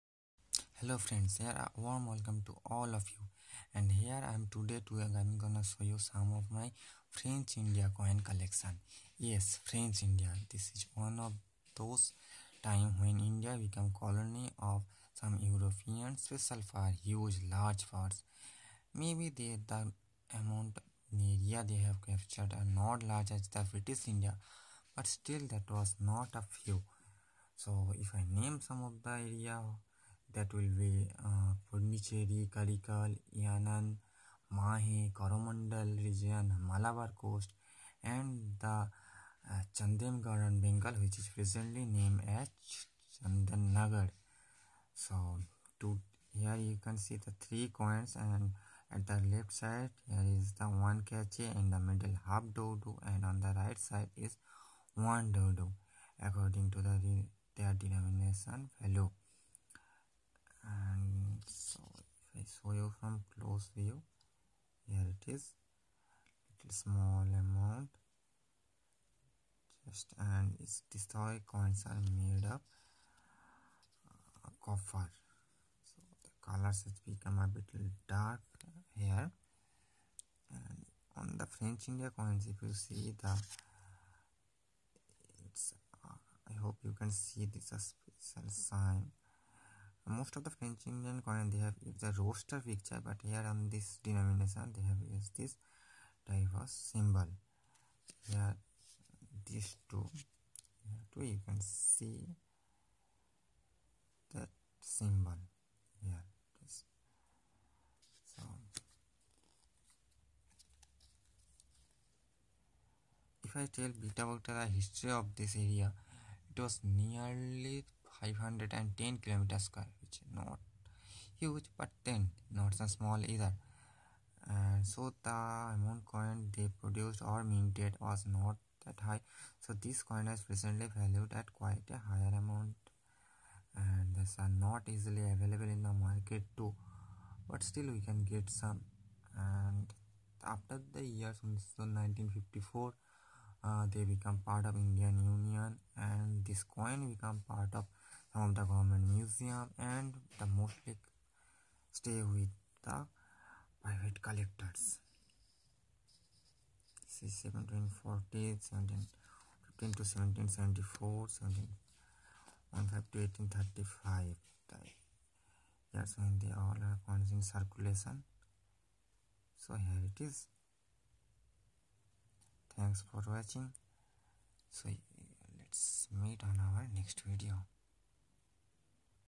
400 Years Old FRENCH INDIA Coins
French India, formally the Établissements français dans l'Inde ("French establishments in India"), was a French colony comprising geographically separate enclaves on the Indian subcontinent. The possessions were originally acquired by the French East India Company beginning in the second half of the 17th century, and were de facto incorporated into the Union of India in 1950 and 1954. The French establishments included Pondichéry, Karikal and Yanaon on the Coromandel Coast, Mahé on the Malabar Coast and Chandernagor in Bengal. French India also included several loges ("lodges", subsidiary trading stations) in other towns, but after 1816, the loges had little commercial importance and the towns to which they were attached came under British administration.

By 1950, the total area measured 510 km2 (200 sq mi), of which 293 km2 (113 sq mi) belonged to the territory of Pondichéry. In 1936, the population of the colony totalled 298,851 inhabitants, of which 63% (187,870) lived in the territory of Pondichéry.

Sournce : Wikipedia